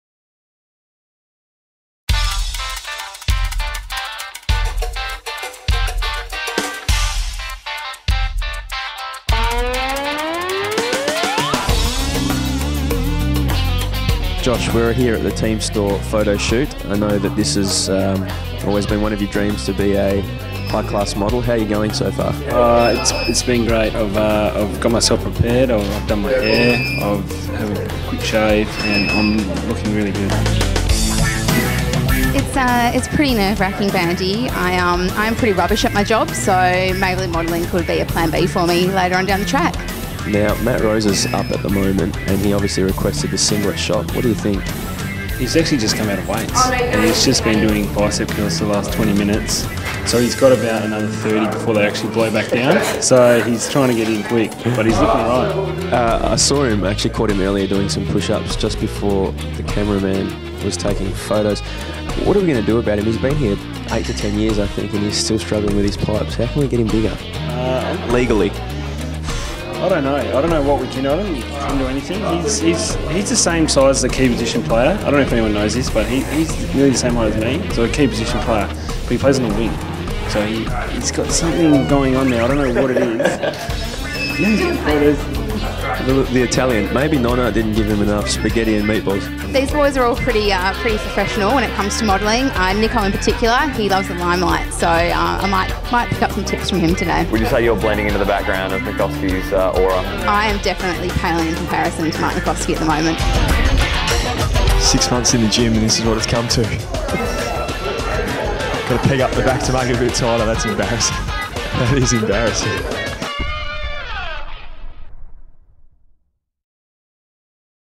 Team Store Photo Shoot - Vol 2
Mark Nicoski, Josh Kennedy and Matt Rosa model the latest clothing apparel from the 2013 Team Store catalogue